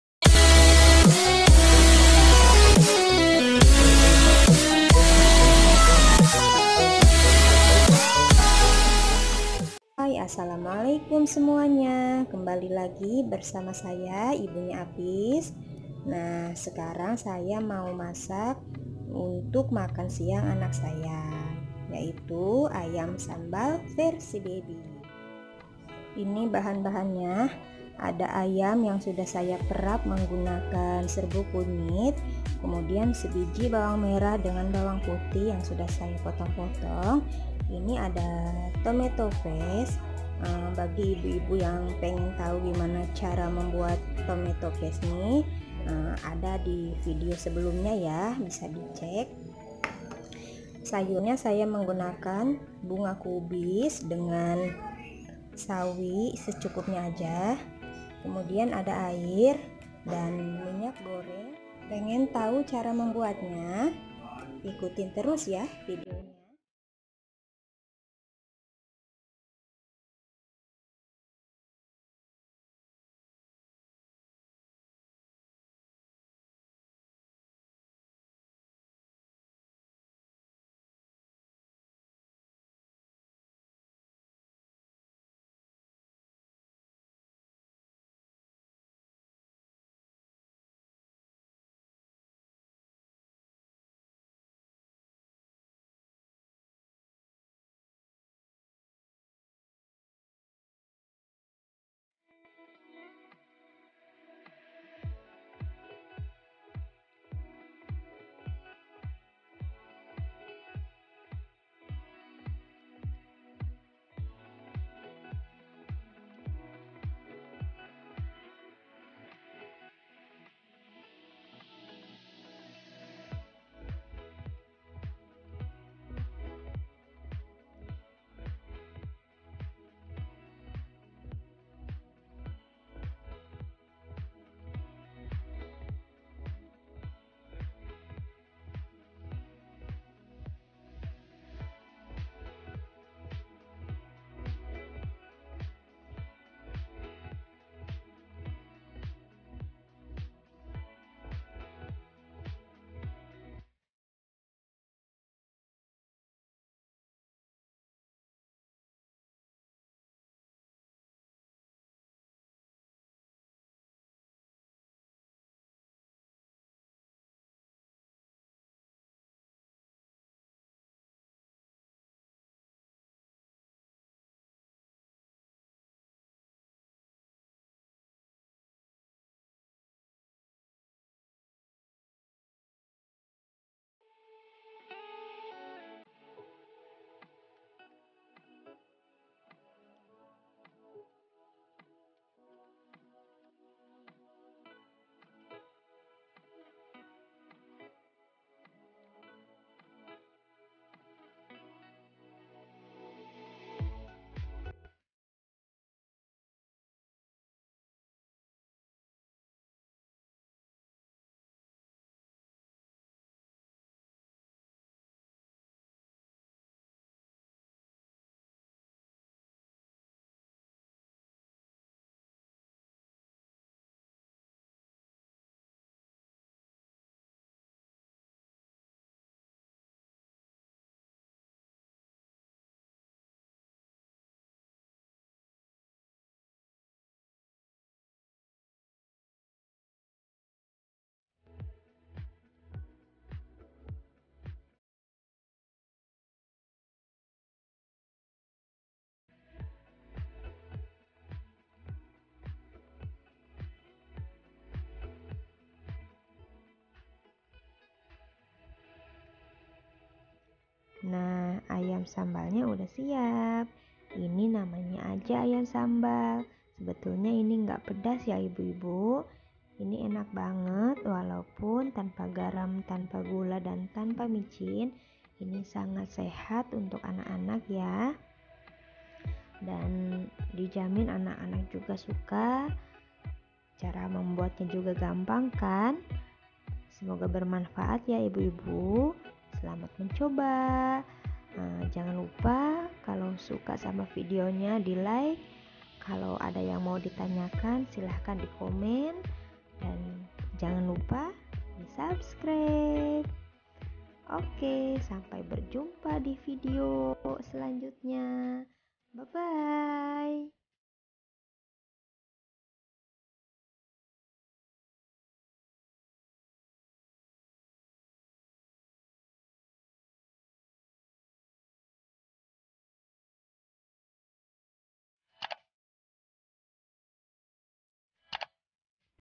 Ayam Sambal Versi Baby | Ide makan Siang siKecil😊
Haii.
Assalamualikum semuanya,
Kali ini saya mau masak untuk makan siang Apis...
Menu yang enak tapi tetap sehat ya
Yaitu:

Ayam Sambal versi Baby

Bahannya :
• Ayam
• Serbuk Kunyit
• Bawang Merah + Bawang Putih
• Tomato Paste (Cara Membuat ada diVideo sebelumnya)
• Bunga Kubis
• Sawi
• Minyak Masak

Cara membuat Nya
silahkan di simak Video nya Ya.